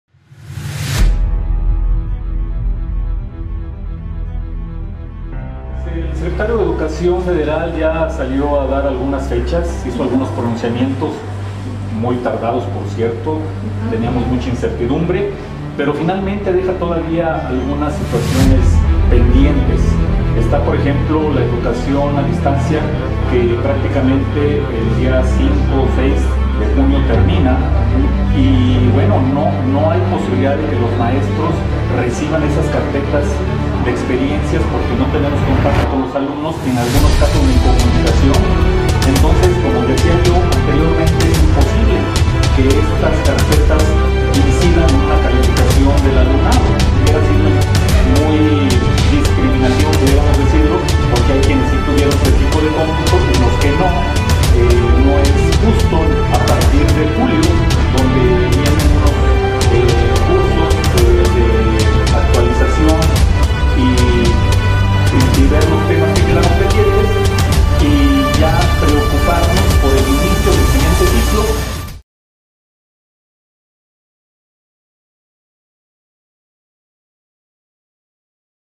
Hay incertidumbre en SGS con las indicaciones al sector educativo para fin de cursos
Local El titular de la Dirección de Educación señaló no hay claridad con respecto al cierre del ciclo escolar y el inicio de inscripciones para el próximo.